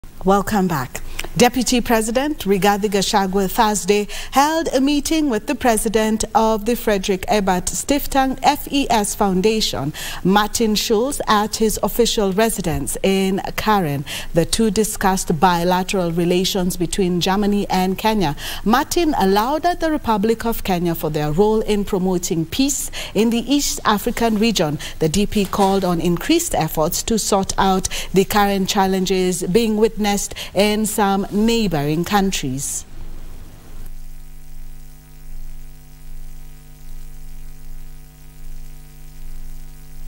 DP Gachagua meets Friedrich Ebert Stiftung boss
Deputy president Rigathi Gachagua Thursday held a meeting with the president of the Friedrich Ebert Stiftung (FES) Foundation Martin Schulz at his official residence in Karen. The two discussed bilateral relations between Germany and Kenya. Martin lauded the republic of Kenya for their role in promoting peace in the East African region. The DP called on increased efforts to sort out the current challenges being witnessed in some neighbouring countries.